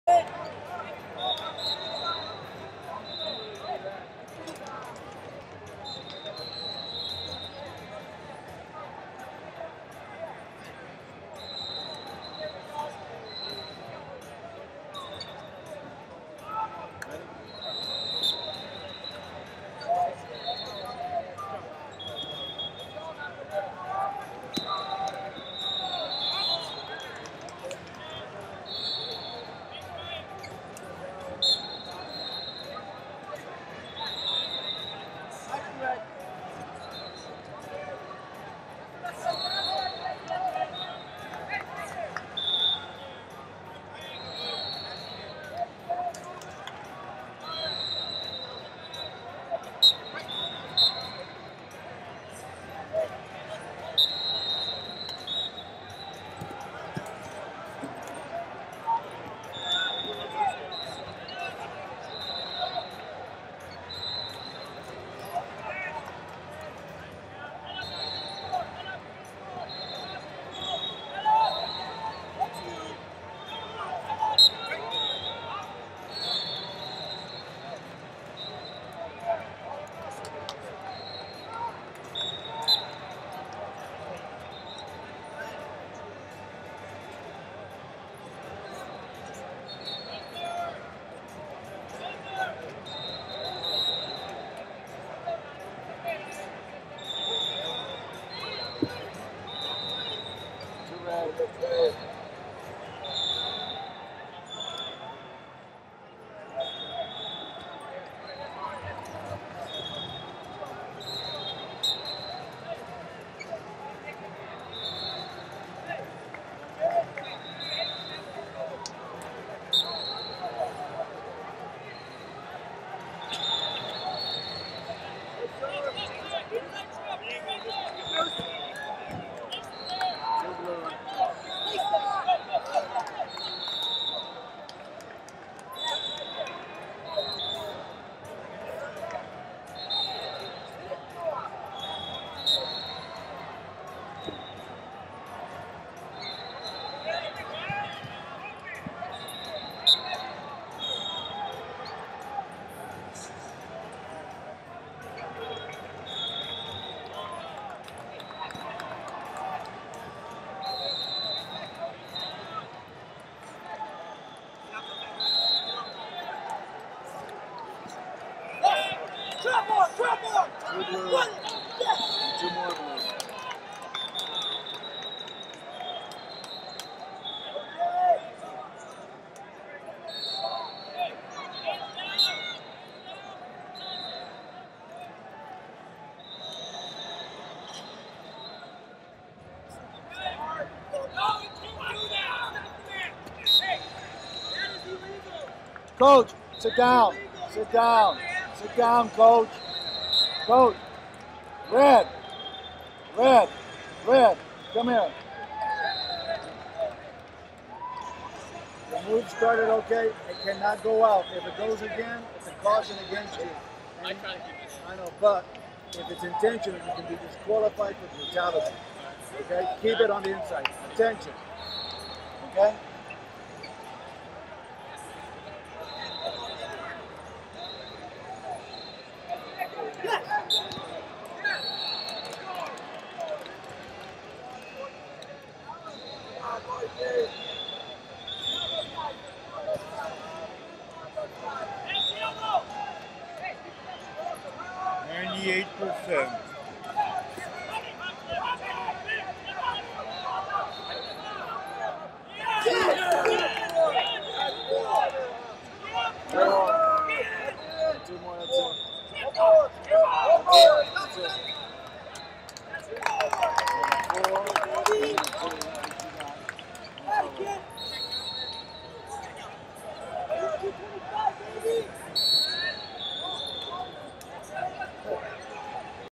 170 Lbs Round Of 16 - Dylan Newsome, Ohio Vs Jared Stricker, Wisconsin E136
FloSports/2021 US Marine Corps Junior And 16U National Championships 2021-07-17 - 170 Lbs Round Of 16 - Dylan Newsome, Ohio Vs Jared Stricker, Wisconsin E136